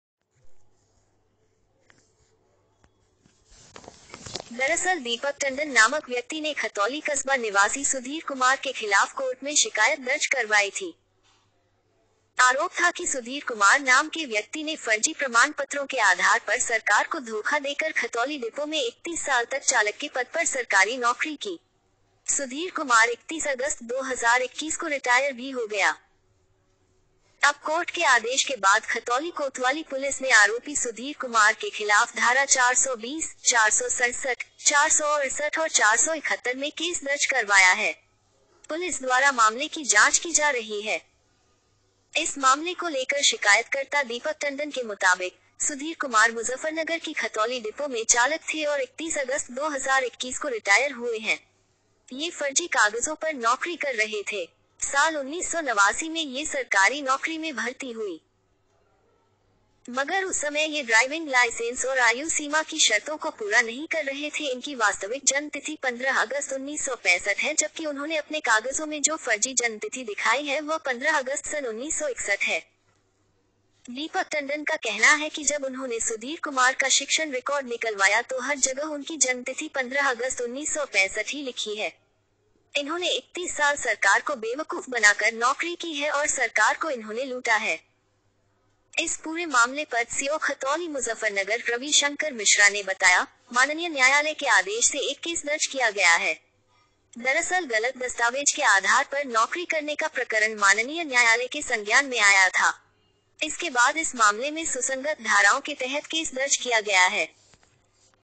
जिलामुजफ्फरनगरफर्जी सर्टिफिकेट से 31 साल तक करता रहा सरकारी नौकरी! रिटायरमेंट होने यूं हुआ केस दर्ज
दरअसल दीपक टंडन नामक व्यक्ति ने खतौली कस्बा निवासी सुधीर कुमार के खिलाफ कोर्ट में शिकायत दर्ज करवाई थी. आरोप था कि सुधीर कुमार नाम के व्यक्ति ने फर्जी प्रमाण पत्रों के आधार पर सरकार को धोखा देकर खतौली डिपो में 31 साल तक चालक के पद पर सरकारी नौकरी की. सुधीर कुमार 31 अगस्त 2021 को रिटायर भी हो गया. अब कोर्ट के आदेश के बाद खतौली कोतवाली पुलिस ने आरोपी सुधीर कुमार के खिलाफ धारा-420, 467, 468 और 471 में केस दर्ज करवाया है. पुलिस द्वारा मामले की जांच की जा रही है.
इस मामले को लेकर शिकायतकर्ता दीपक टंडन के मुताबिक, सुधीर कुमार मुजफ्फरनगर की खतौली डिपो में चालक थे और 31 अगस्त 2021 को रिटायर हुए हैं. यह फर्जी कागजों पर नौकरी कर रहे थे. साल 1989 में ये सरकारी नौकरी में भर्ती हुई. मगर उस समय ये ड्राइविंग लाइसेंस और आयु सीमा की शर्तों को पूरा नहीं कर रहे थे  इनकी वास्तविक जन्म तिथि 15 अगस्त 1965 है जबकि उन्होंने अपने कागजों में जो फर्जी जन्मतिथि दिखाई है वह 15 अगस्त सन 1961 है.

दीपक टंडन का कहना है कि जब उन्होंने सुधीर कुमार का शिक्षण रिकॉर्ड निकलवाया तो हर जगह उनकी जन्मतिथि 15 अगस्त 1965 ही लिखी है. इन्होंने 31 साल सरकार को बेवकूफ बना कर नौकरी की है और सरकार को इन्होंने लूटा है.
इस पूरे मामले पर (सीओ खतौली-मुजफ्फरनगर) रवि शंकर मिश्रा ने बताया, “ माननीय न्यायालय के आदेश से एक केस दर्ज किया गया है. दरअसल गलत दस्तावेज के आधार पर नौकरी करने का प्रकरण माननीय न्यायालय के संज्ञान मे आया था. इसके बाद इस मामले में सुसंगत धाराओं के तहत केस दर्ज किया गया है.“